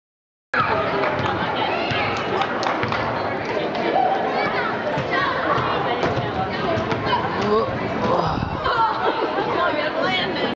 Toe Touch Basket Toss
This was our first night doing them and as you can hear, my boyfriend Greg was a bit nervous and kind of grunts and lets a sigh of relief as he films haha. :D